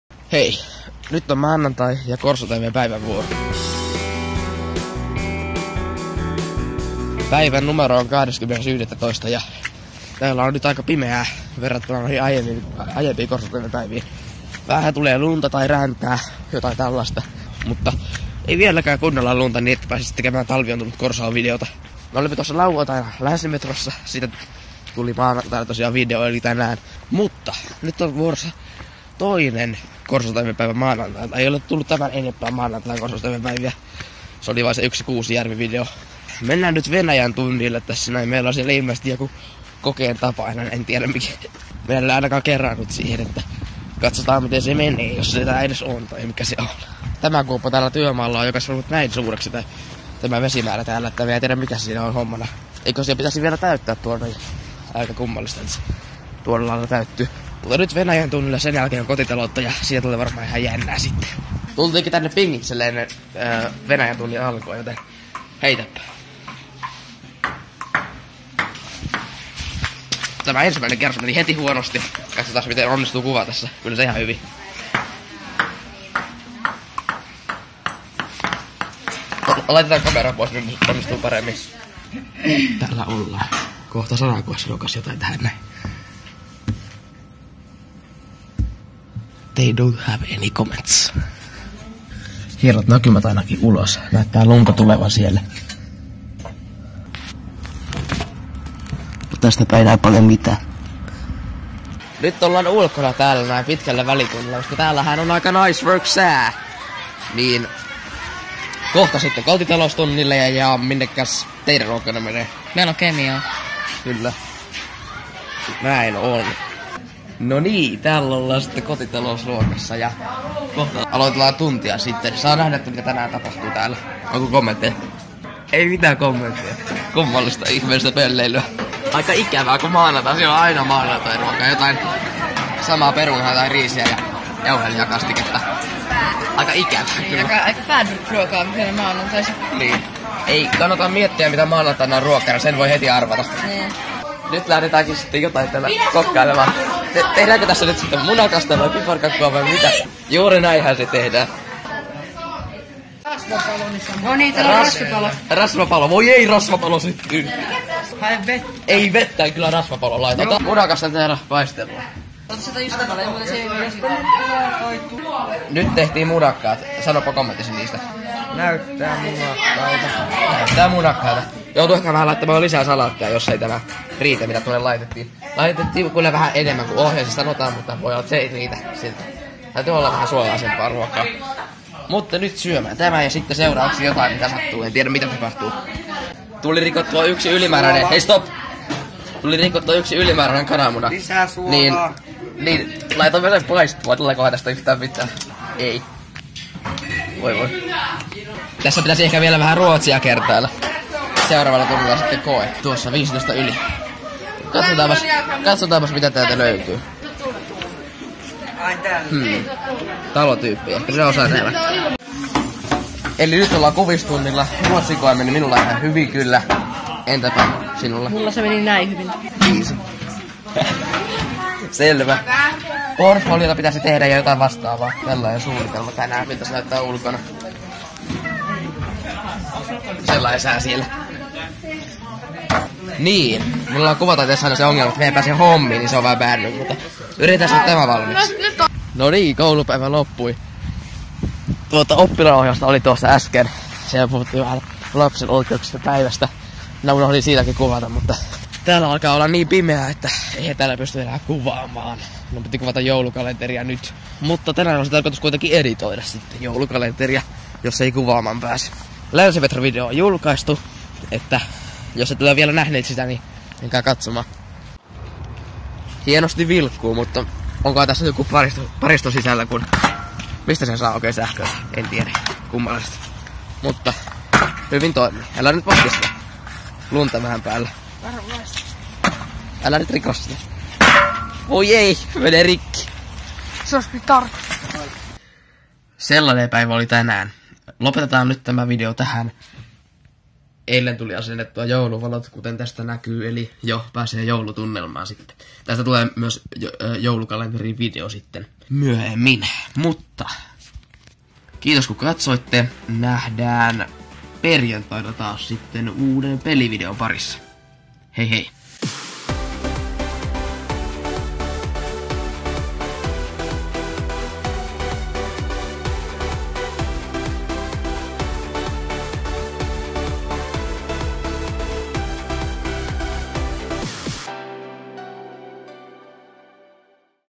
Maanantairuokaa - KorsoTV:n päivä 20.11.2017
Tämä on vasta toinen KorsoTV:n päivä -video maanantailta. Kokkasimme kotitaloustunnilla munakasta ja teimme ruotsin kokeen. Mahtui päivään myös hieman työmaavalon vilkkumista.